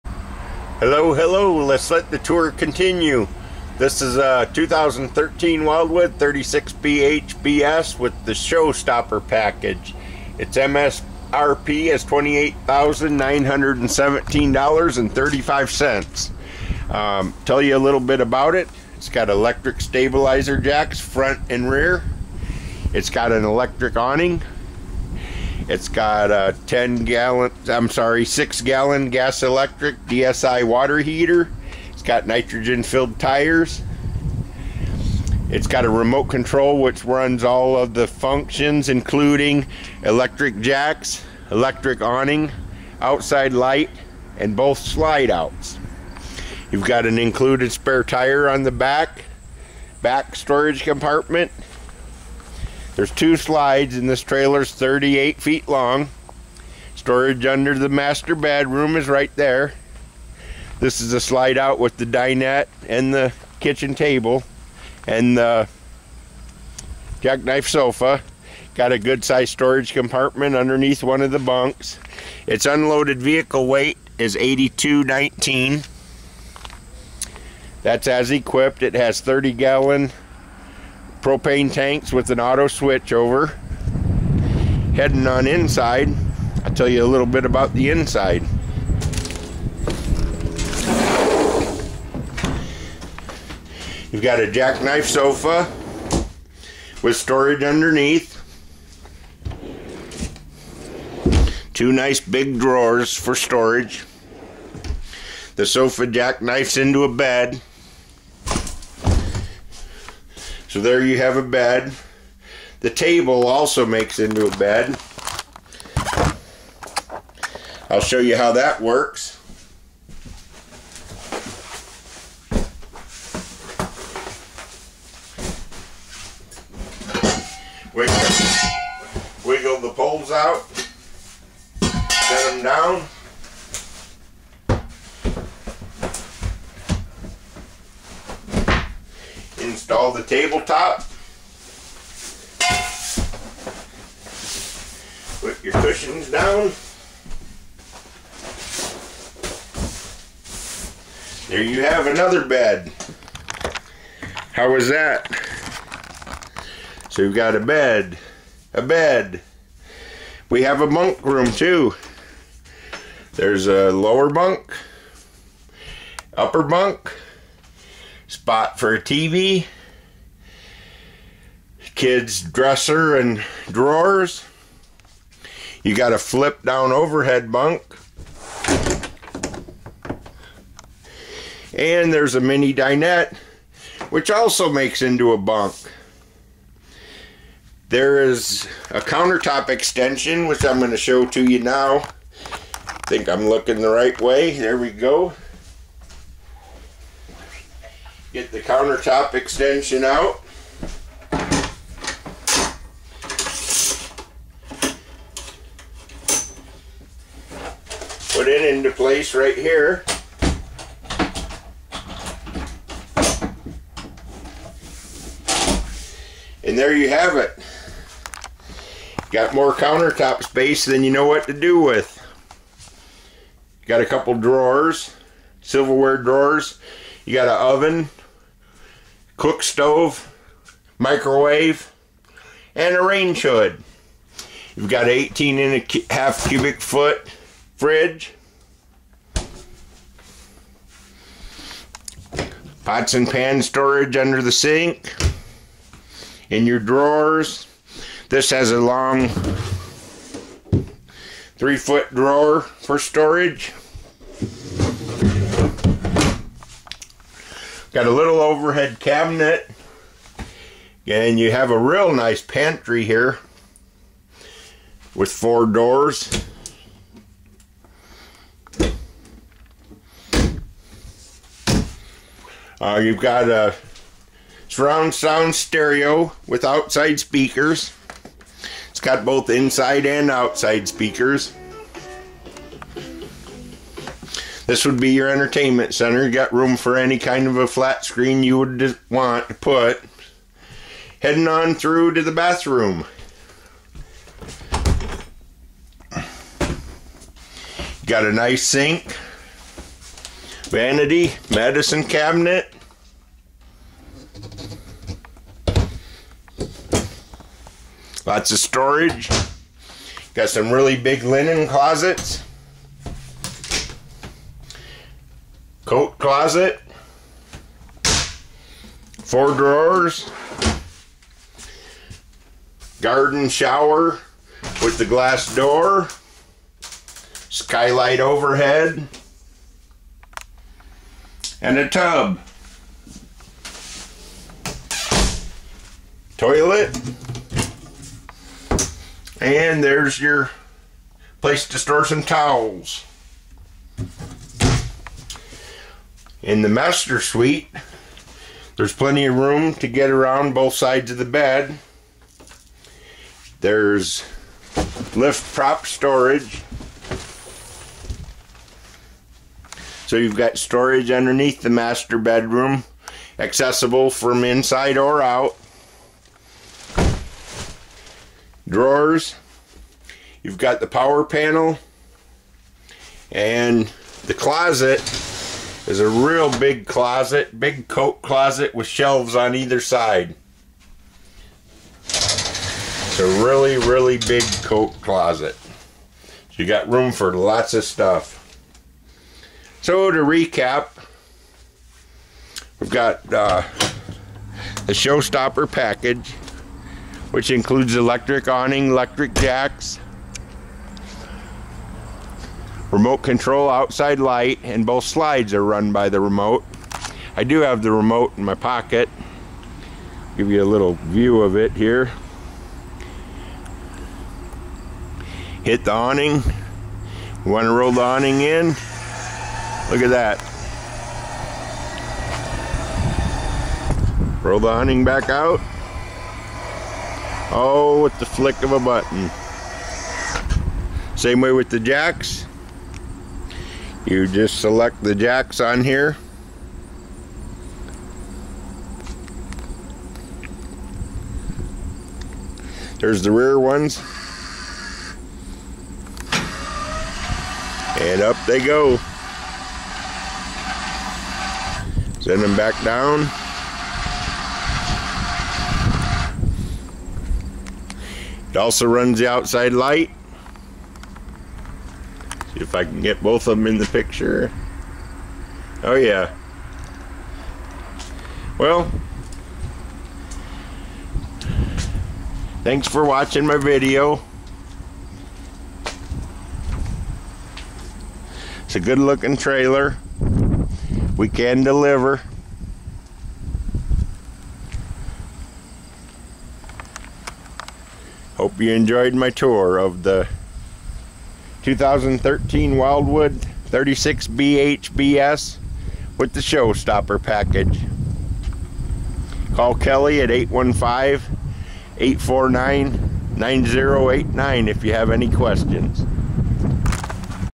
WILDWOOD 36BHBS SHOWSTOPPER INTERIOR TOUR
This is an Inside Tour of my last remaining 2013 Wildwood 36BHBS with the Showstopper Package. In the previous video I demonstrate the Remote Control Stabilizer Jacks, the Remote Control Slideouts, the Remote Control Awning, and last but not least the Remote Control Outside Light. MSRP is $28,917.35.